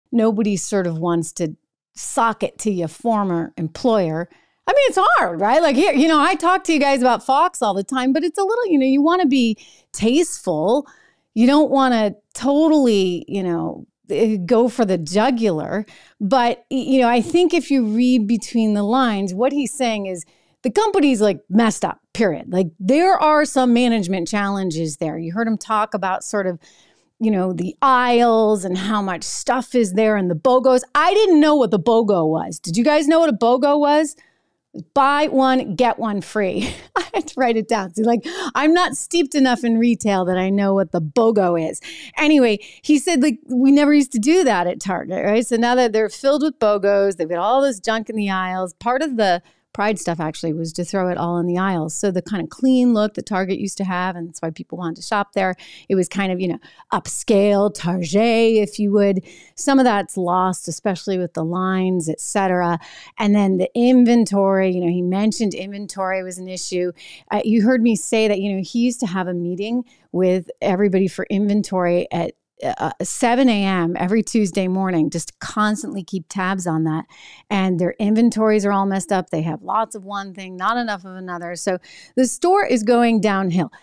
Messy Aisles, Theft: Target’s Faces More than Just Boycott
In This Clip, Trish Regan discusses the multiple issues Target stores now face regarding quality control and inventory. Is Target losing its exclusive draw?